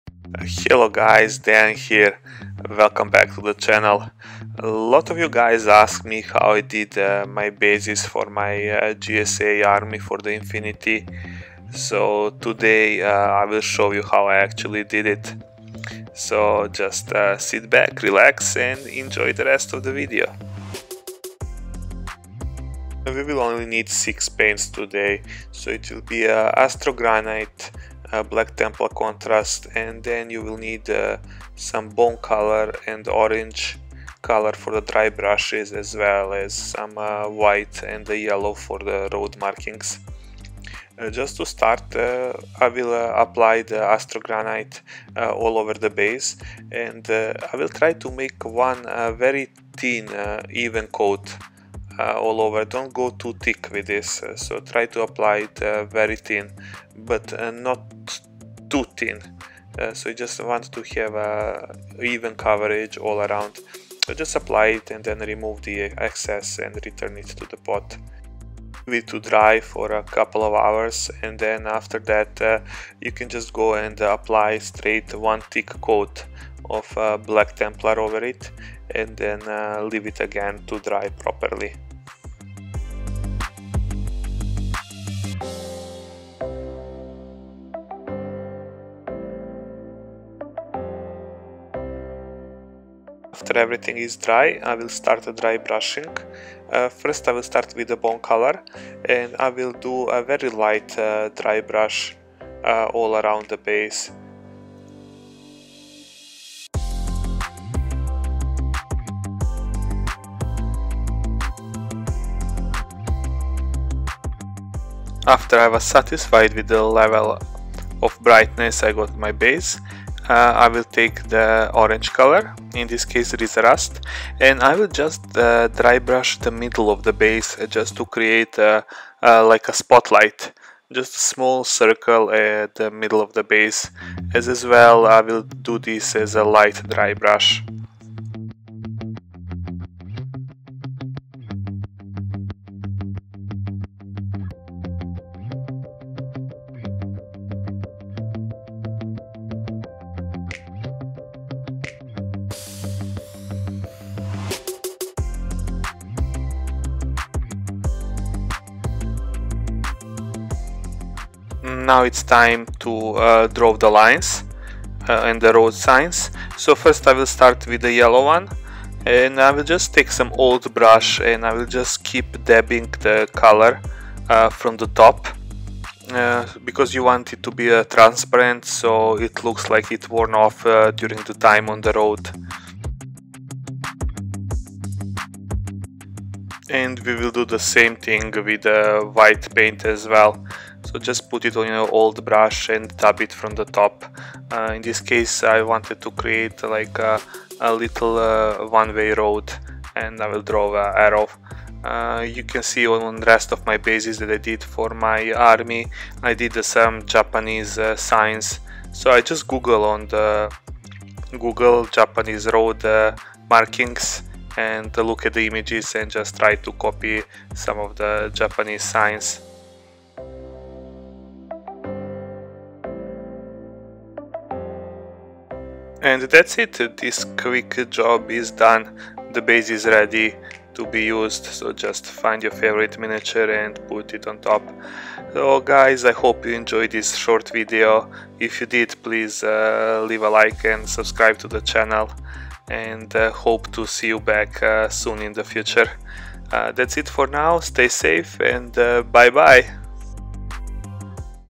How to Make and Paint: Urban Asphalt Base
Today i will show you how i did urban asphalt base for my JSA army.

Used products
Astrogranite
Black Templar Contrast
Terminatus Stone
Ryza Rust
White Scar
Avarland Sunset